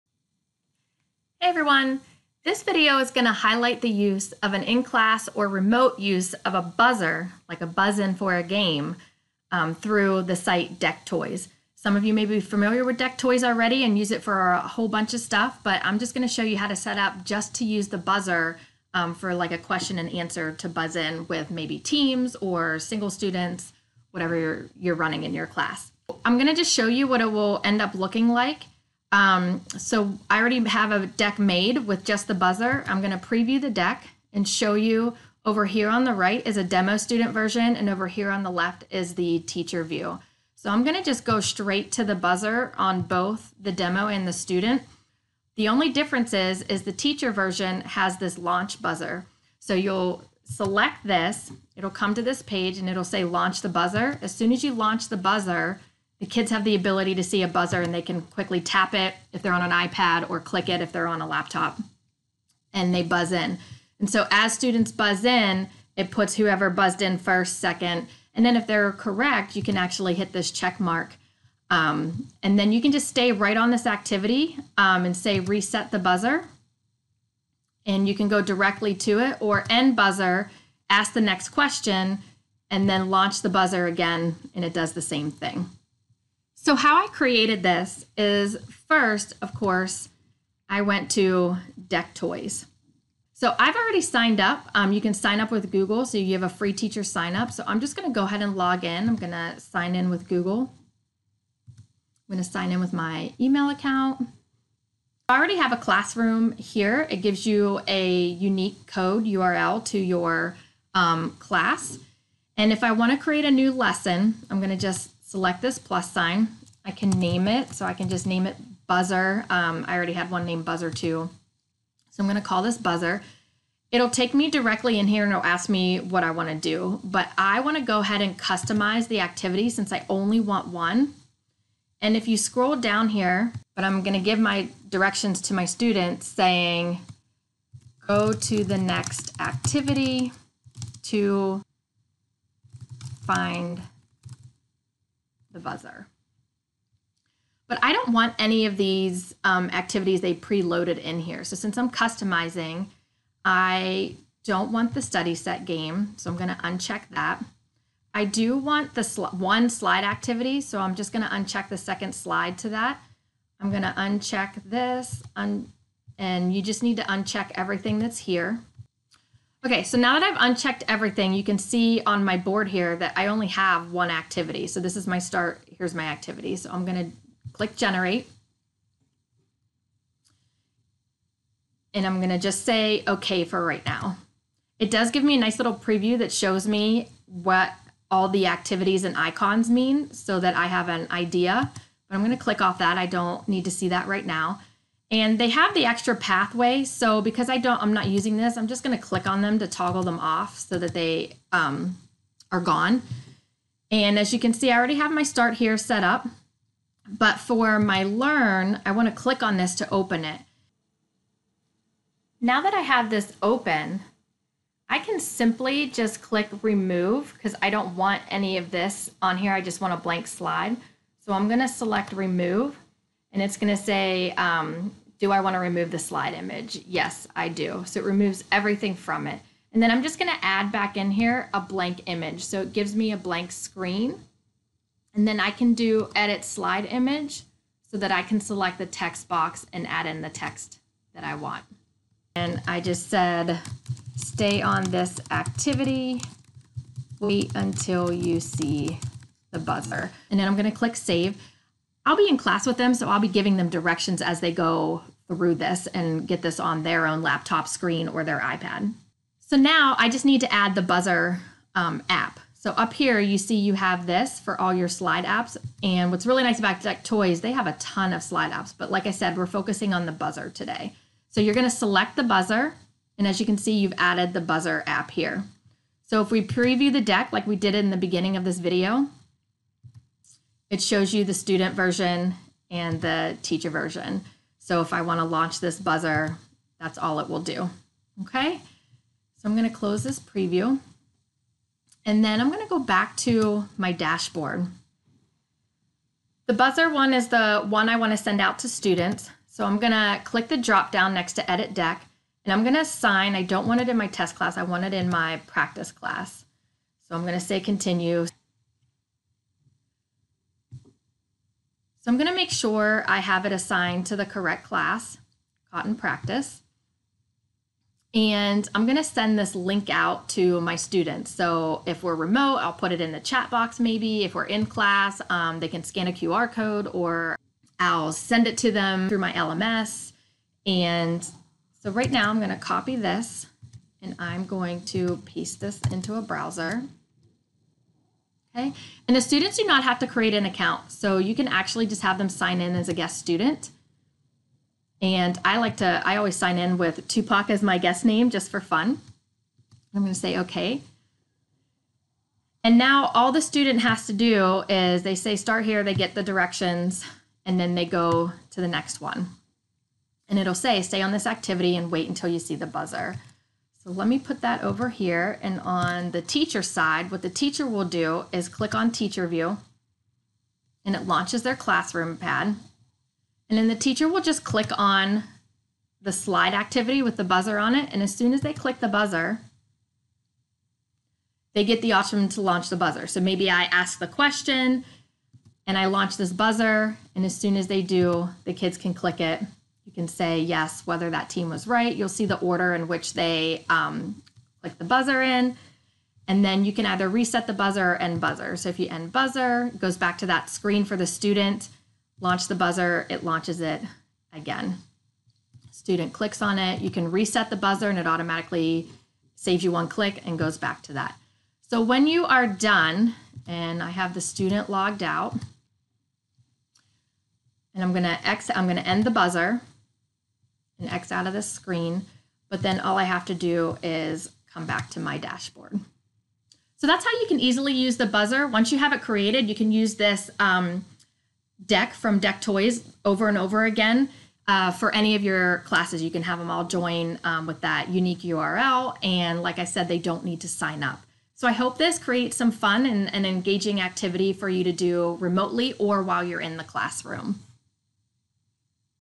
Create a Buzzer "buzz-in" Activity in Deck Toys
Learn how to create a digital "buzz-in" activity in Deck Toys. Now, you can play review games with teams (or individually) while you are remote or in a classroom.